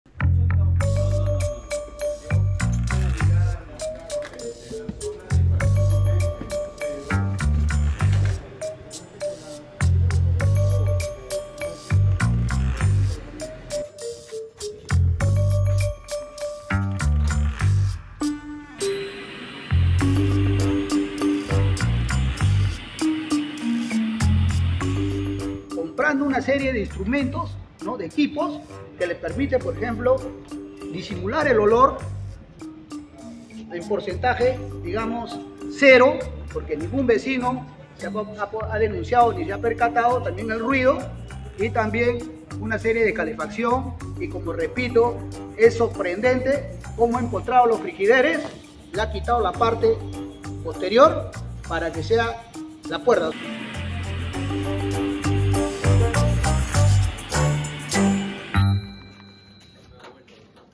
Punya Pintu Rahasia untuk Tanaman Ganja, Pria Dibekuk Polisi
Ini bukan pintu Doraemon ya, gengs!🚪

Seorang pria di Peru berakhir di balik jeruji besi karena apes ketahuan menyembunyikan 106 tanaman ganja di ruangan tersembunyi di rumahnya: di belakang kulkas!

Seperti halnya di Indonesia, tanaman marijuana merupakan barang haram di Peru.